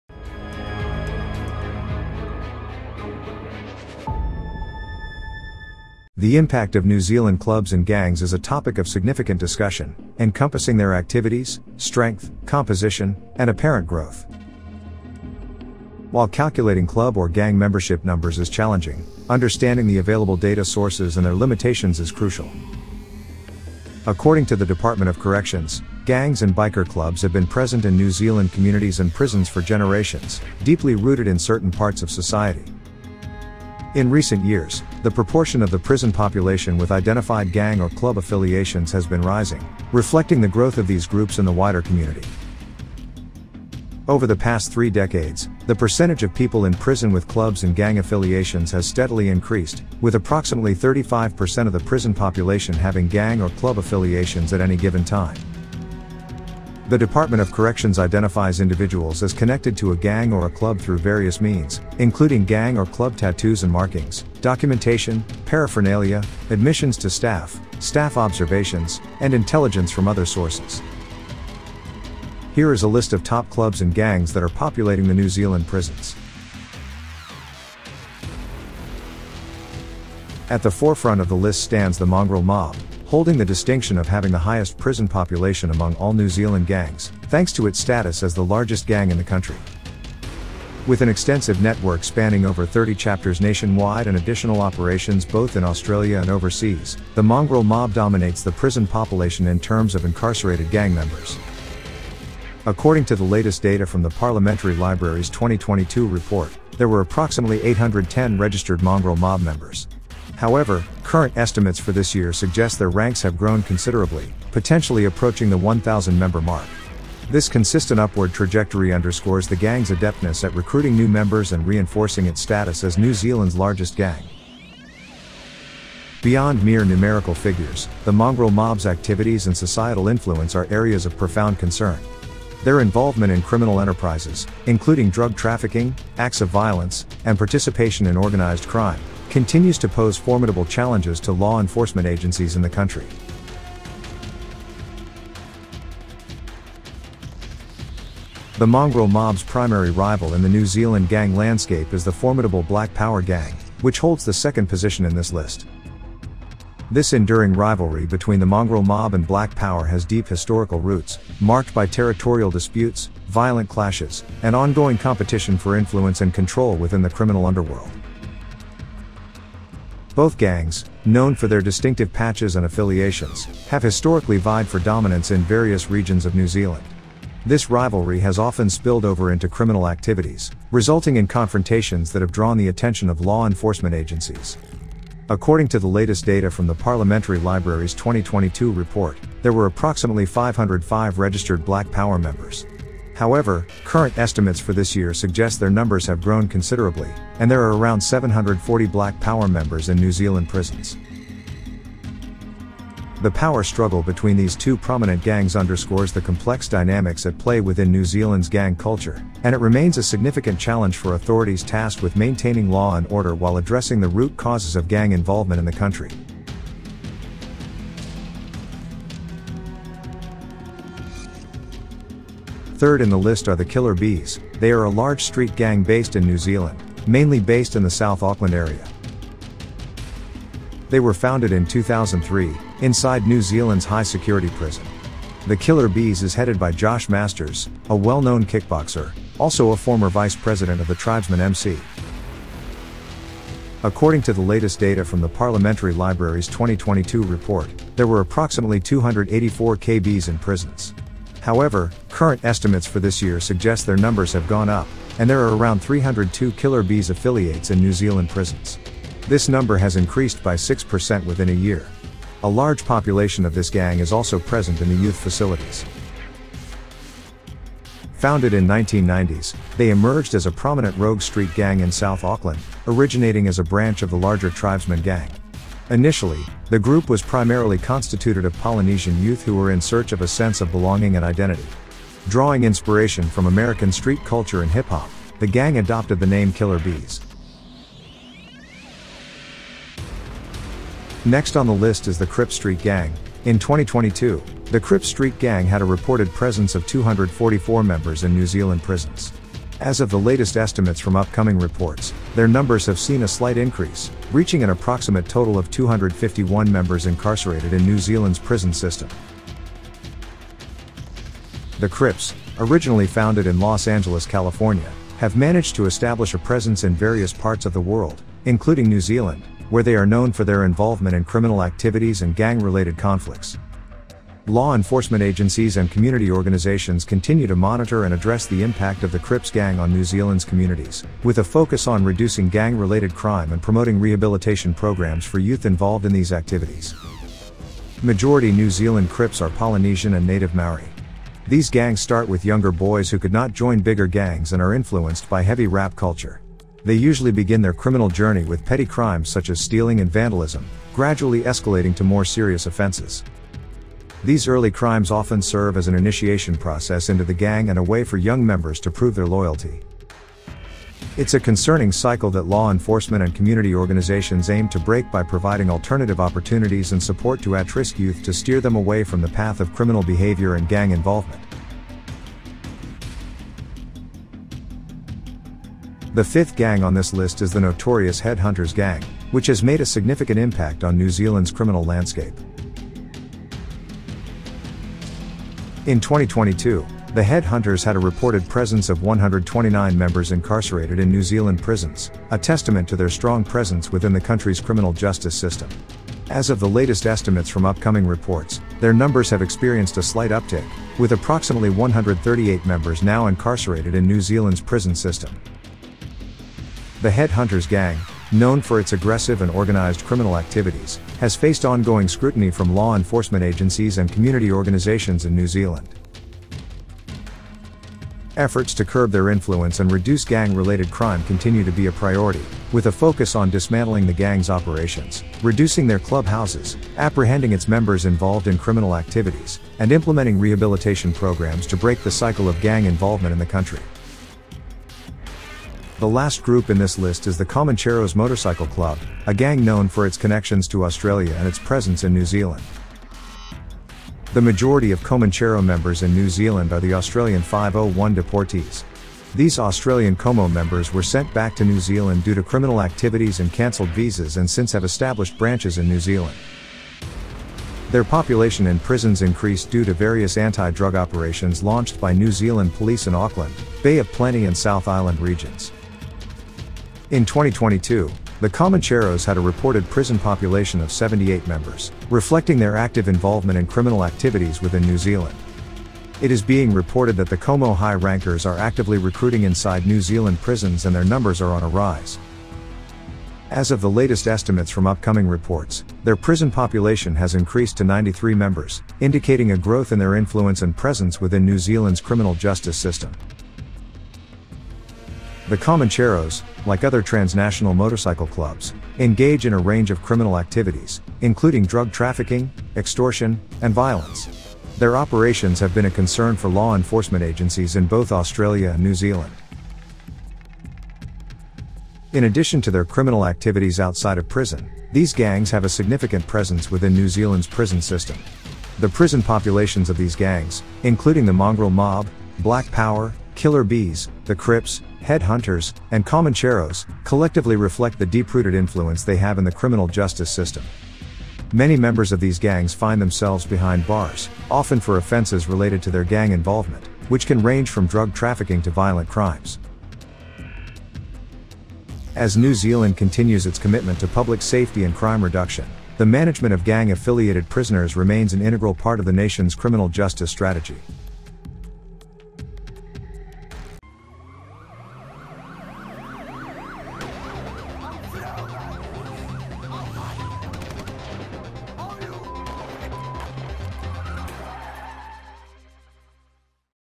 Top 6 prison gangs of New Zealand
The impact of New Zealand clubs and gangs is a topic of significant discussion, encompassing their activities, strength, composition, and apparent growth. While calculating club or gang membership numbers is challenging, understanding the available data sources and their limitations is crucial.

According to the Department of Corrections, gangs and biker clubs have been present in New Zealand communities and prisons for generations, deeply rooted in certain parts of society.

Over the past three decades, the percentage of people in prison with clubs and gang affiliations has steadily increased, with approximately 35% of the prison population having gang or club affiliations at any given time. The Department of Corrections identifies individuals as connected to a gang or a club through various means, including gang or club tattoos and markings, documentation, paraphernalia, admissions to staff, staff observations, and intelligence from other sources.
Here is a list of top clubs and gangs that are populating the new zealand prisons.
Follow Grid Sparta on